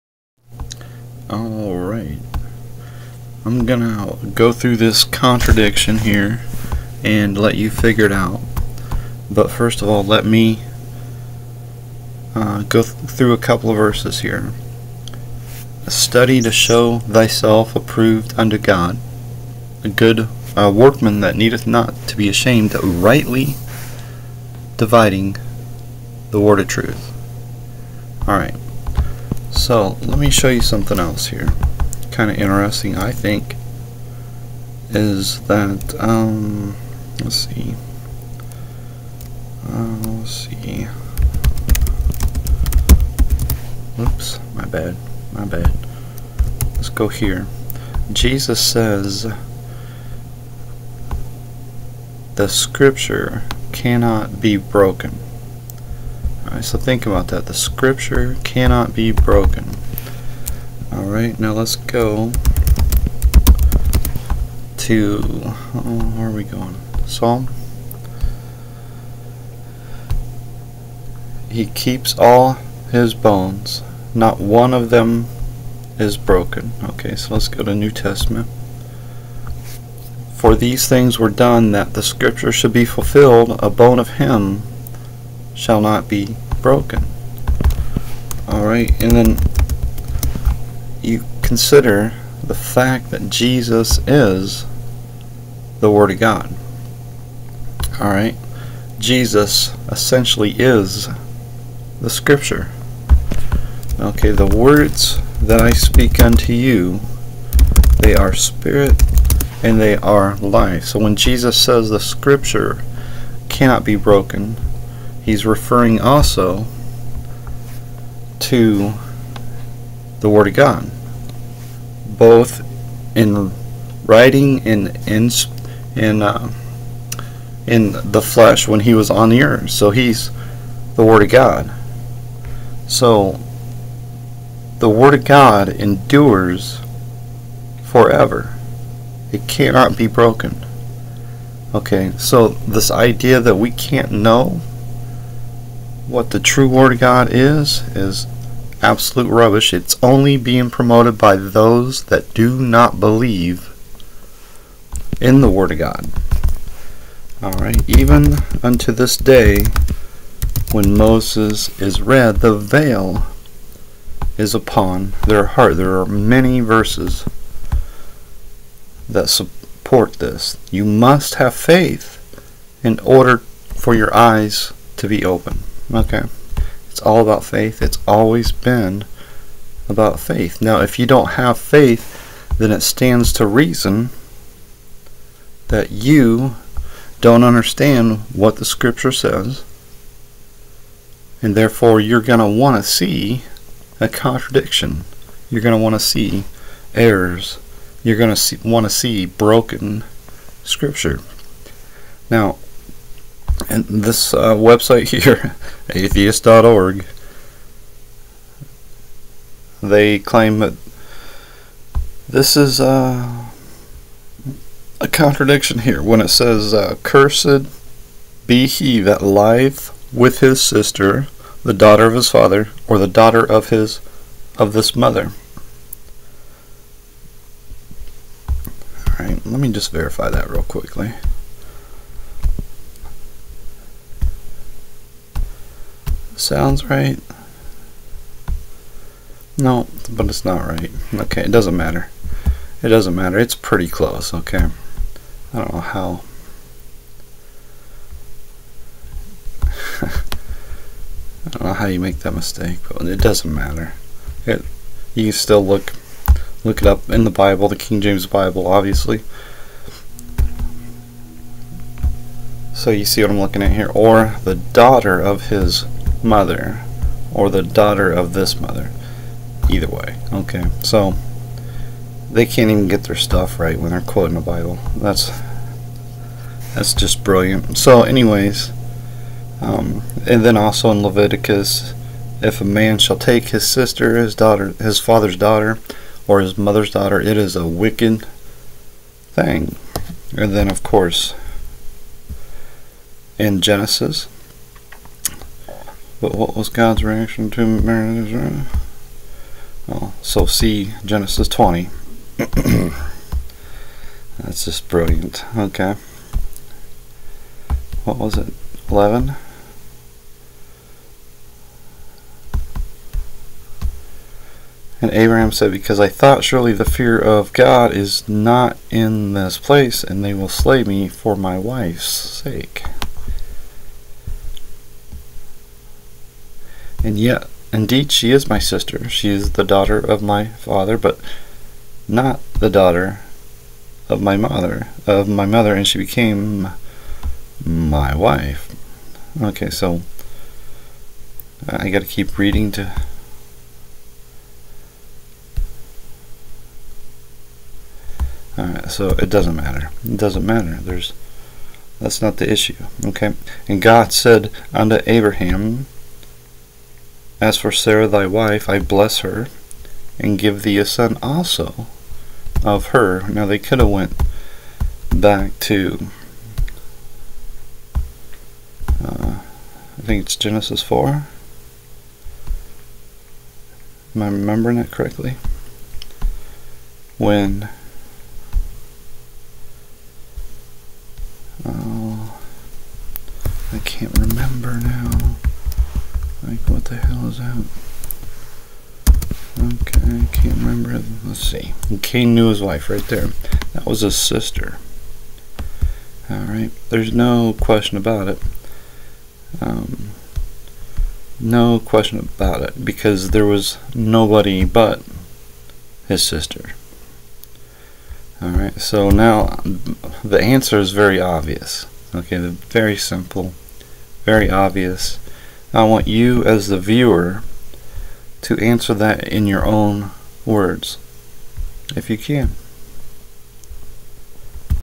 Incest Bible Contradiction
Leviticus 20:17 And if a man shall take his sister, his father's daughter, or his mother's daughter, and see her nakedness, and she see his nakedness; it is a wicked thing; and they shall be cut off in the sight of their people: he hath uncovered his sister's nakedness; he shall bear his iniquity.